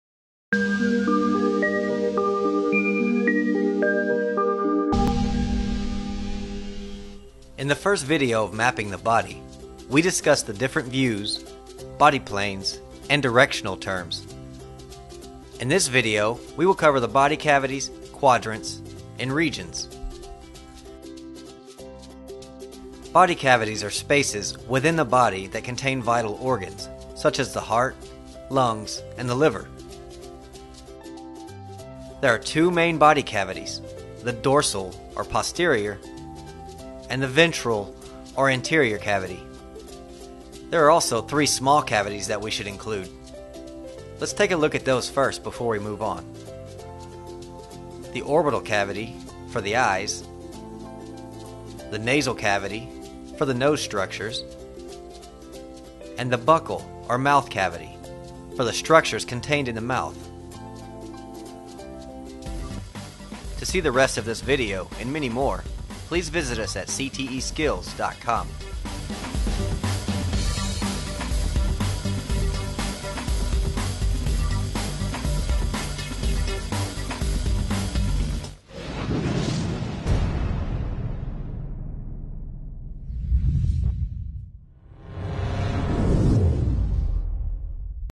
Mapping the Body Pt. 2 (Cavities, Quadrants, Regions)
See the entire video @ Anatomy

Mapping of the body is extremely important to anyone in the Health Sciences or medical fields. Whether you are a nurse aide or a surgeon, you need to understand the body in order to care for a patient properly. Views, body planes and directional terms will be the first of two videos in this Mapping the Body series. In part 2 we will cover the Body Cavities, Quadrants and regions.